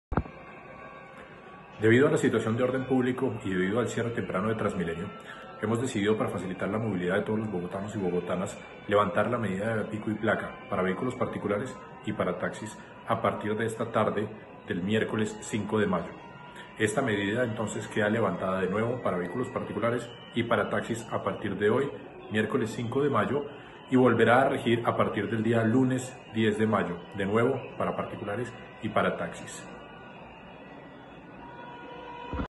SE LEVANTA LA MEDIDA DE PICO Y PLACA
Declaraciones en video del Secretario Distrital de Movilidad, Nicolás Estupiñán.